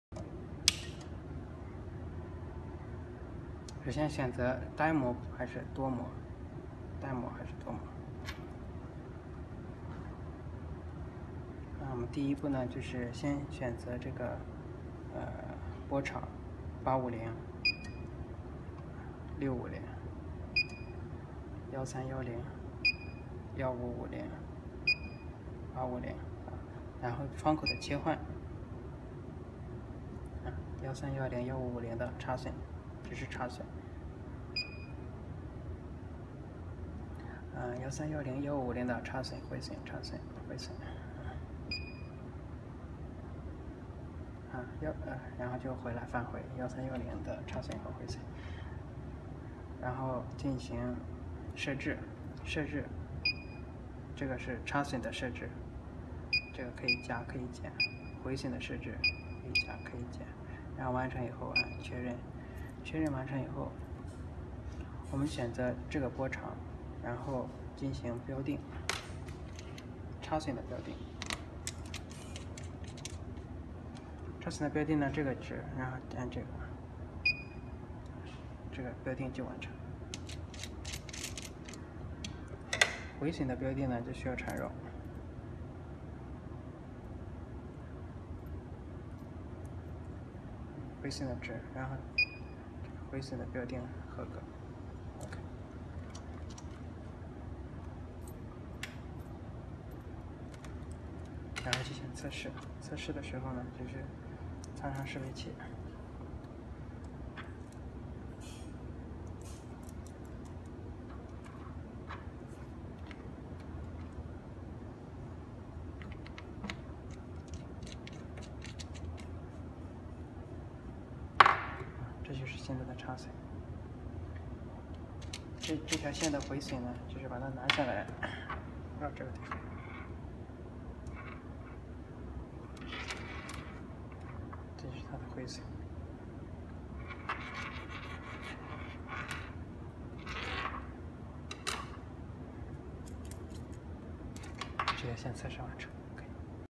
How to use the Insertion loss and return loss testing machine for both singemode and multimode
this Insertion loss and return loss testing machine can test IL for both SM and MM, and RL for SM